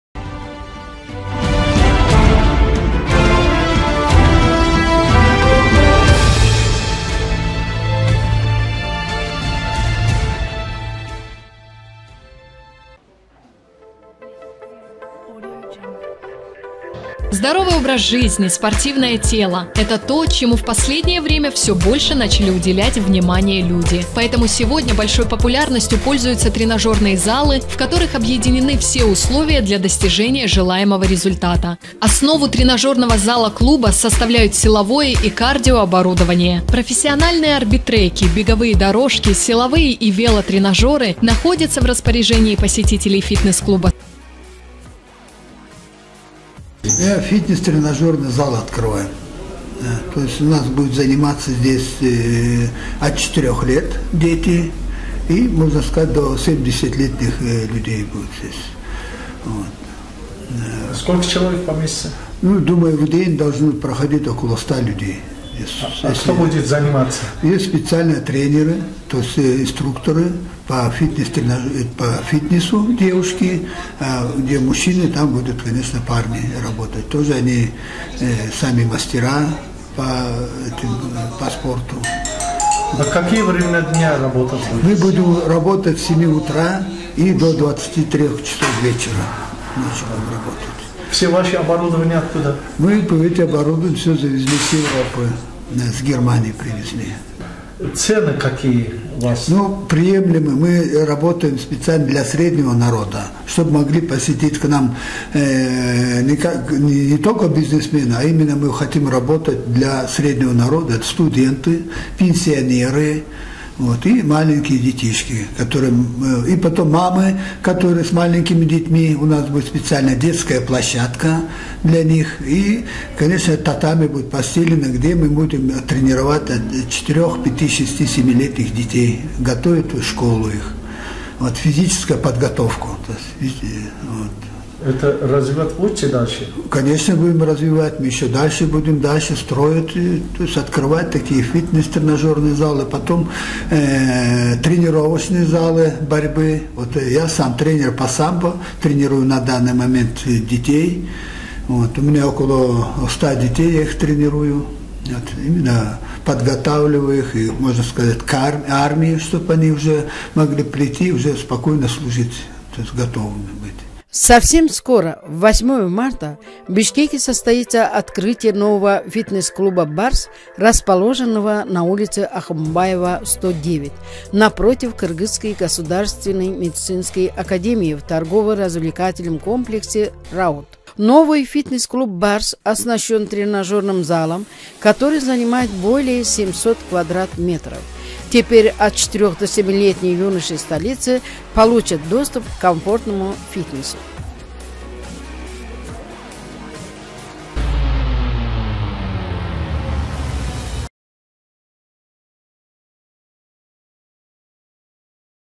В Бишкеке состоится открытие нового фитнес клуба «Барс»
Бишкек (Golden Bridge News) Совсем скоро в Бишкеке состоится открытие нового фитнес клуба «Барс», расположенного на улице Ахунбаева 109, напротив Кыргызской государственной медицинской академии в Торгово-развлекательном комплексе «Раут».
Новый фитнес-клуб «арс» оснащен тренажерным залом, который занимает более 700 м.кв. Теперь 4-7 леетние юноши столицы получат доступ к комфортному фитнесу.